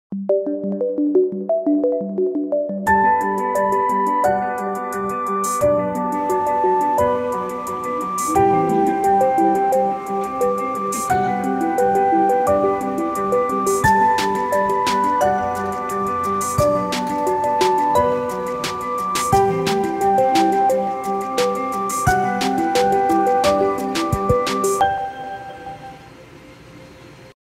From Sketch to Artwork: Scottish Castle (Art Inspired by Paul Clark)
I painted this Scottish castle using one of Paul Clark's tutorial in his watercolor channel. :)